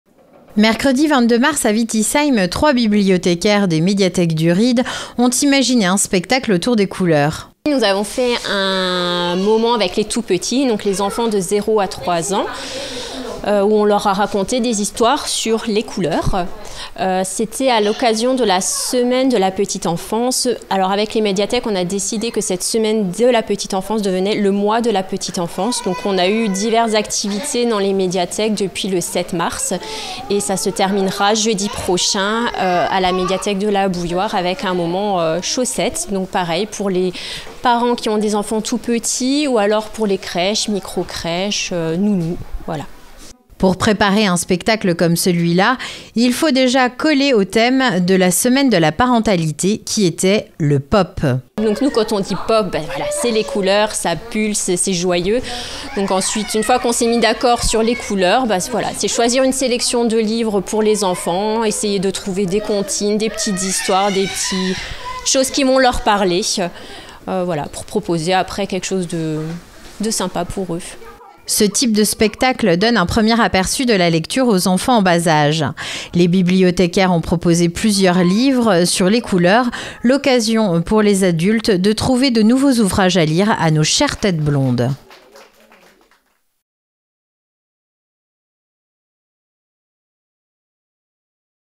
Spectacle Médiathèque pour les touts petits
Les médiathèques du Ried ont proposé plusieurs animations aux tout petit tous le mois de Mars. Nous sommes allés à Wittisheim pour en connaître la raison.